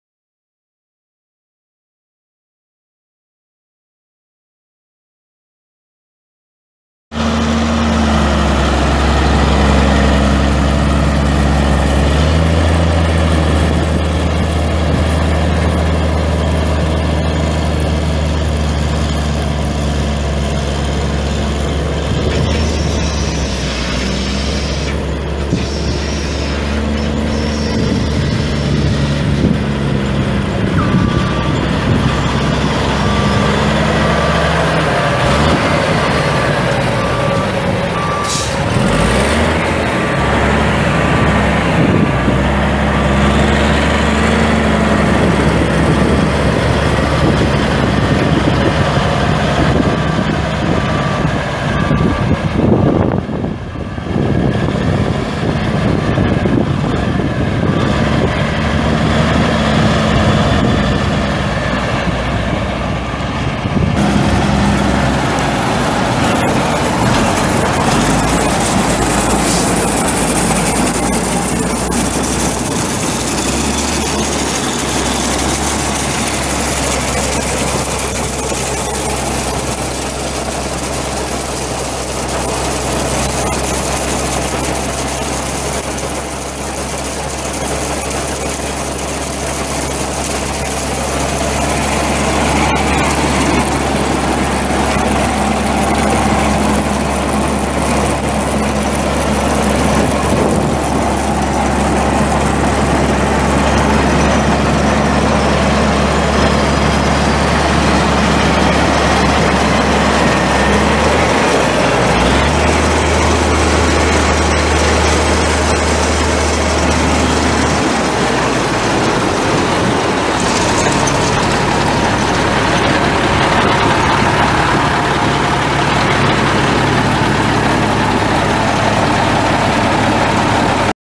1980 Peterbilt 353S23 manure spreader truck for sale at auction | bidding closes April 25, 2018
1980 Peterbilt 353S23 manure spreader truck
81,123 miles on odometer
Caterpillar diesel engine
Manual transmission
Tandem axle
Differential lock
Heat
Manual windows and locks
Chelsea PTO
18'8"L x 7'W steel manure spreader bed
48" side height
(2) beaters
Spring suspension
Air brakes
11R24.5 tires
Kansas title